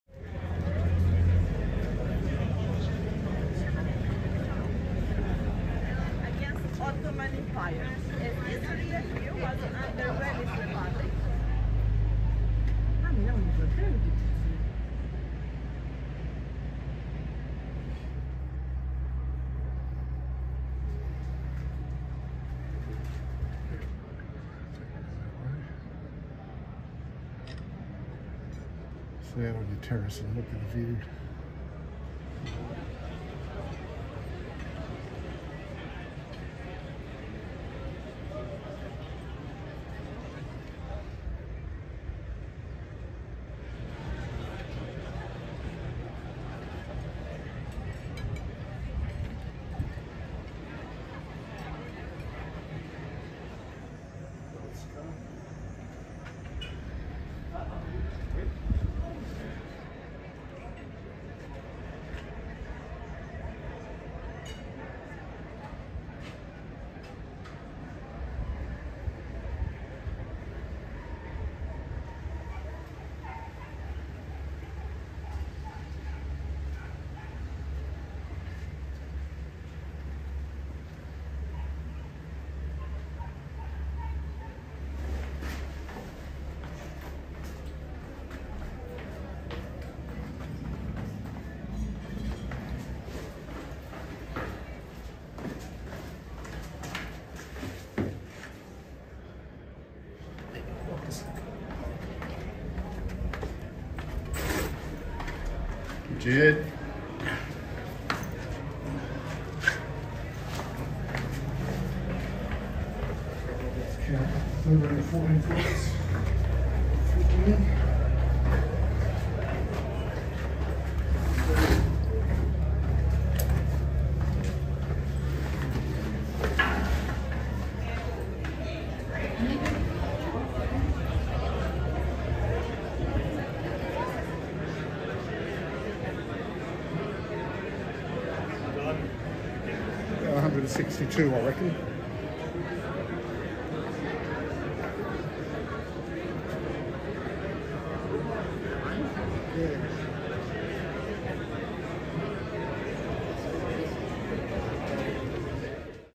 St Nicholas' Bell Tower, Perast
The tower was built in 1691 and is the most recognizable landmark in Perast. The bell tower has three bells and a Venetian clock (1730).